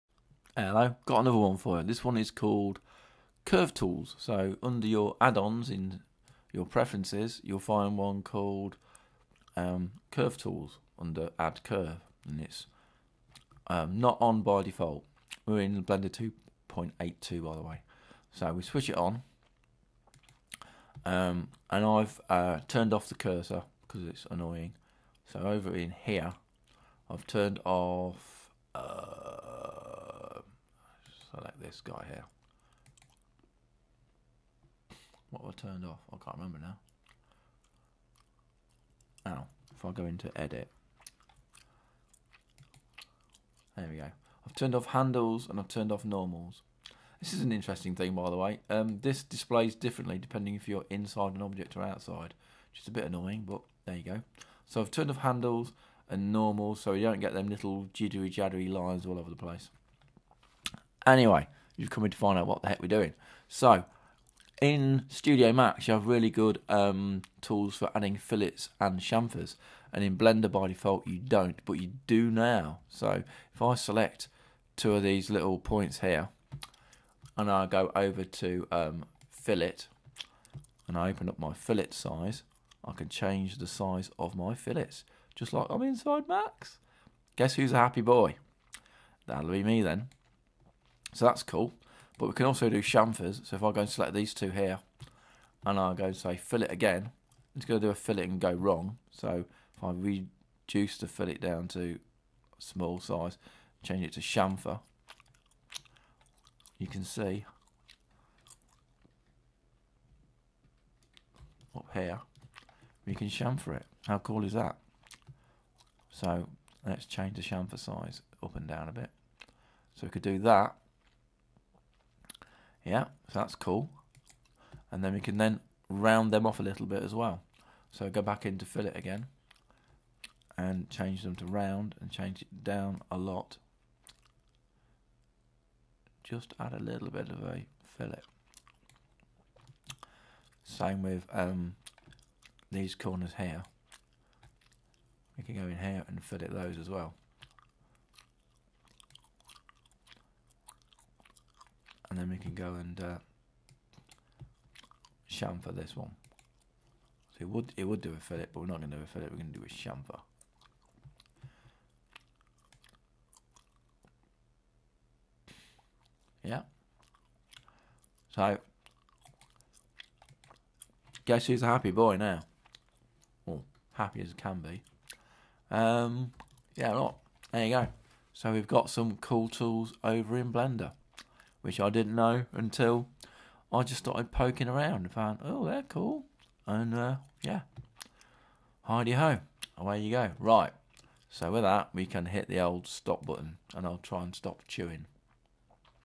Blender 2 82 curve tools add ion
so now you can chamfer and fillet points on curves  just like in 3ds max.